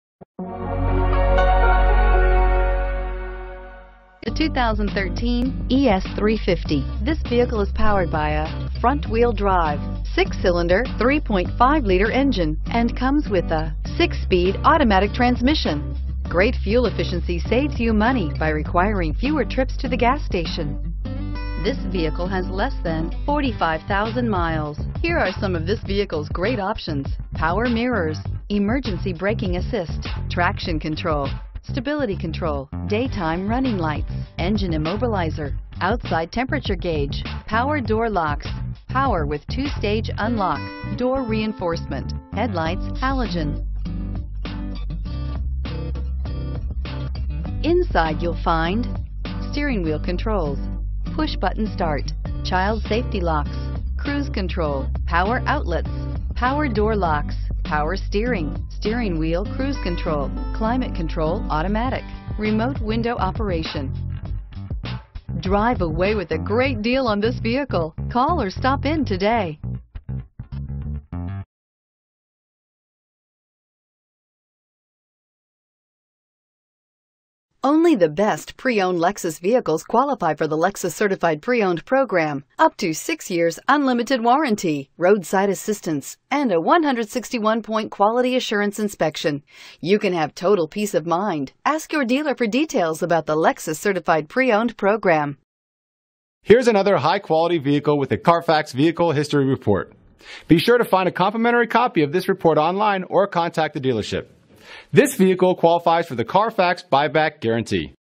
2013 Lexus ES 350 for sale in Raleigh NC
2013 Lexus ES 350 for sale in Raleigh NC - Stock # 26308A
Year: 2013
Exterior: Silver Lining Metallic
Interior: Black
Miles: 41675
MSRP: N/A
Engine: 3.5L V6 DOHC Dual VVT-i 24V
Transmission: Automatic
Address:
VIN: JTHBK1GG0D2028854